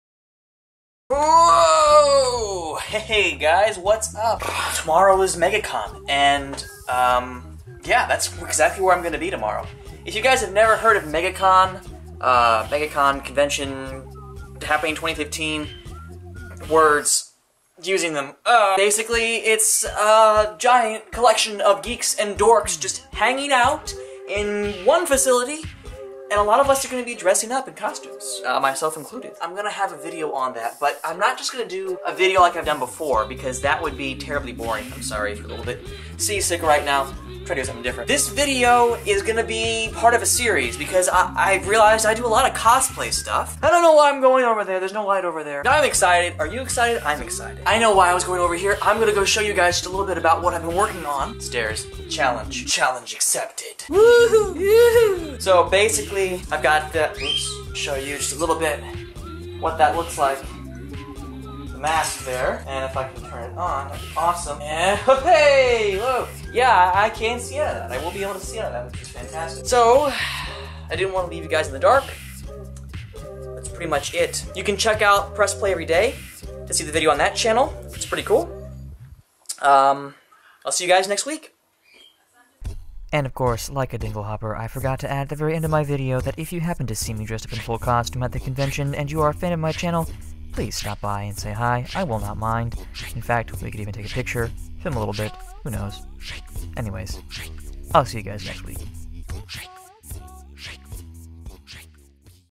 Find me at MegaCon! | Stafeezy
So I decided to film a quick update to let you guys know who I'll be where I'll be tomorrow! (Spider-Man Noir just in case you are unfamiliar with the Spider-verse)

MegaCon 2015 here I come!